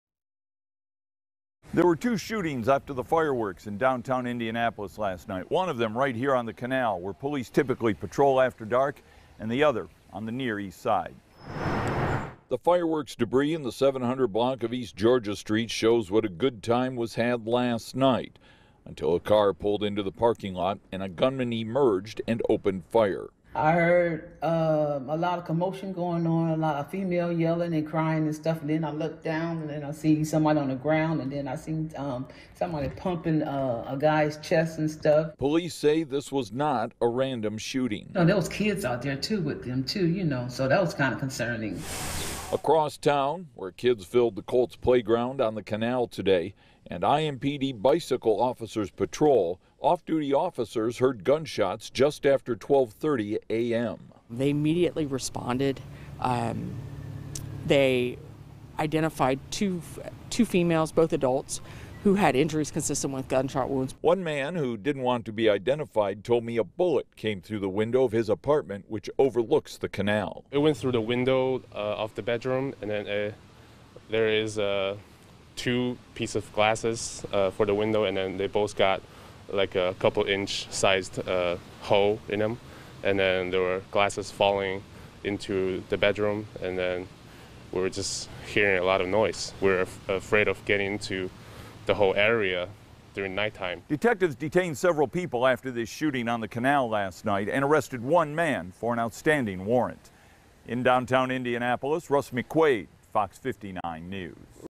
1 dead, 3 hurt in downtown Indianapolis shootings
1 dead, 3 hurt in downtown Indianapolis shootings